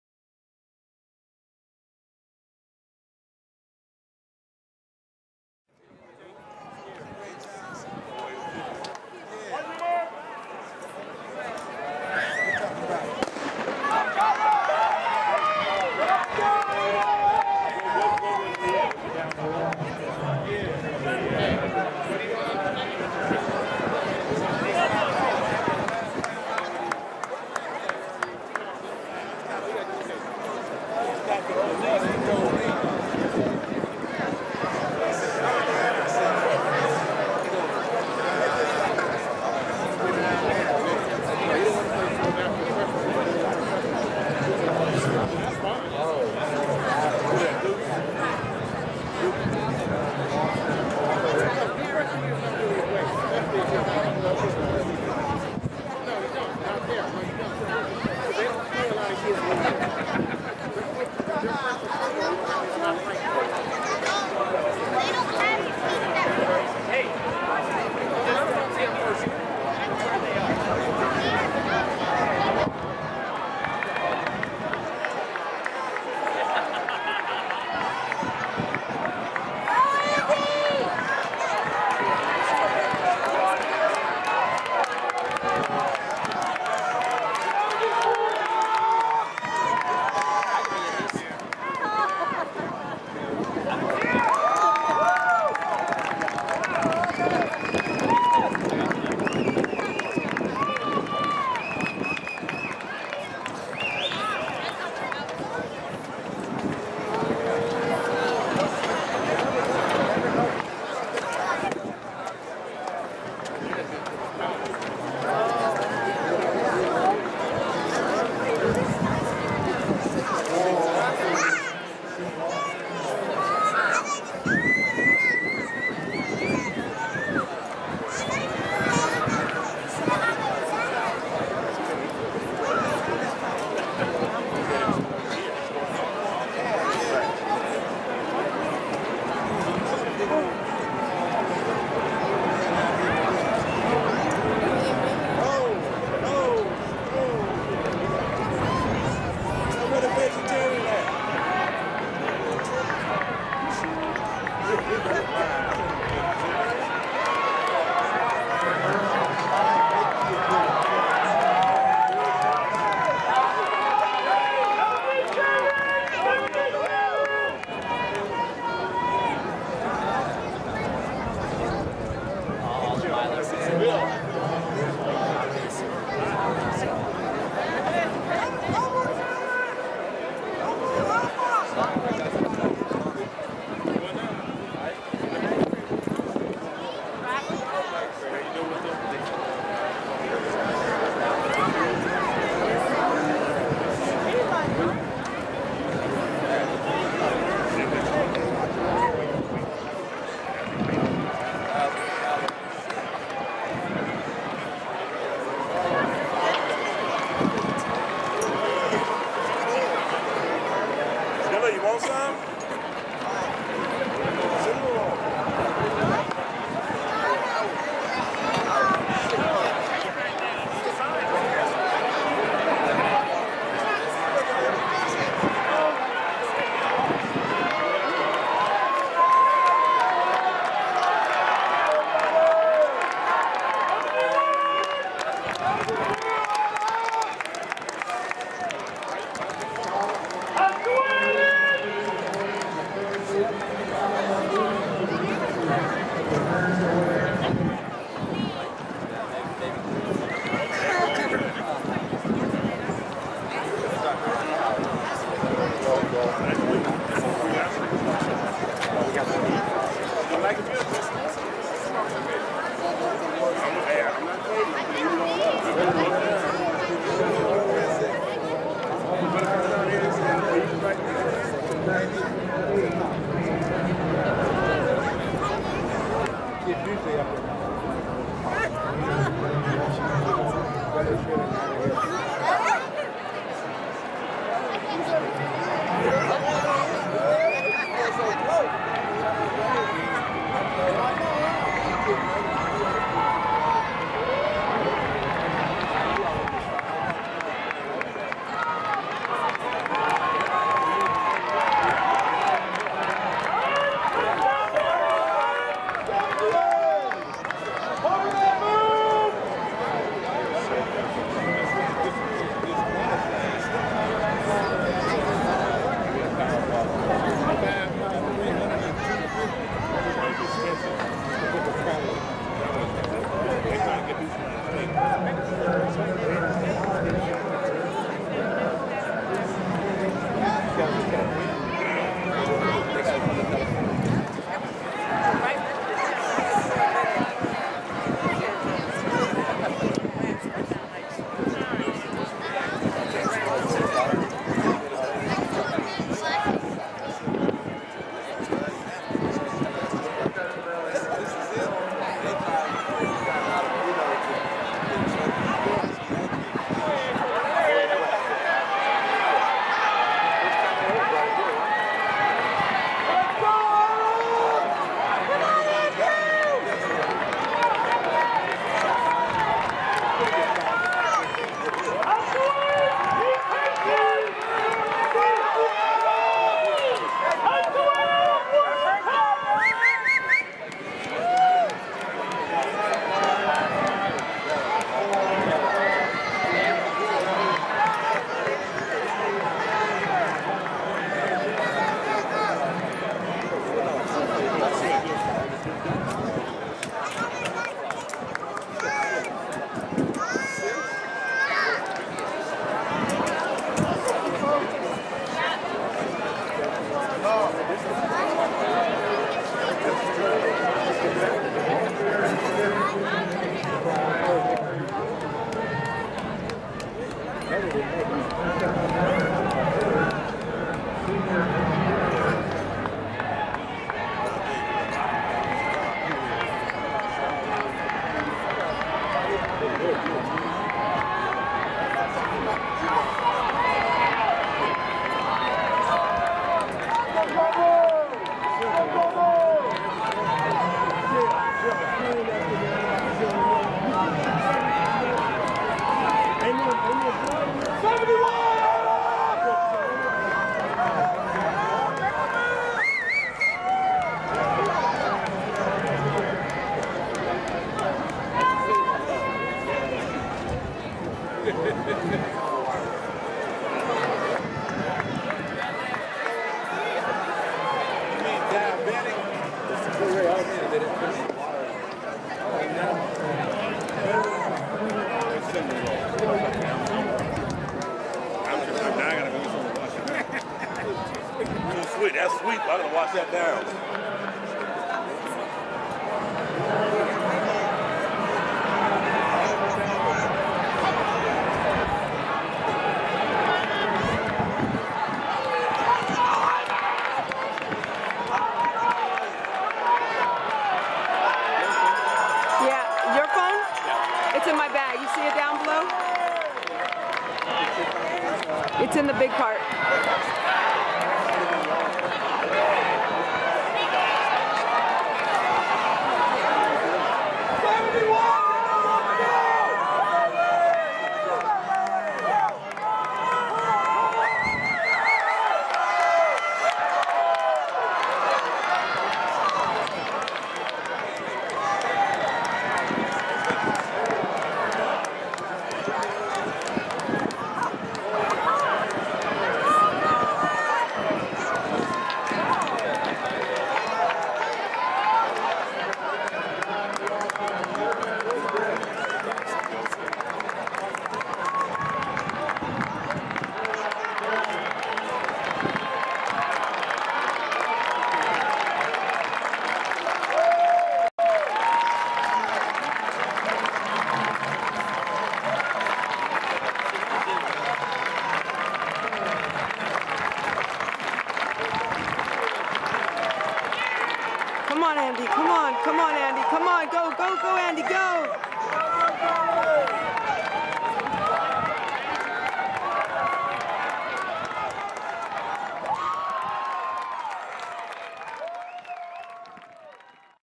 2013 NCS Meet of Champions, Boys 3200
NCS Meet of Champions held at Cal's Edwards Stadium on Saturday, May 26, 2013.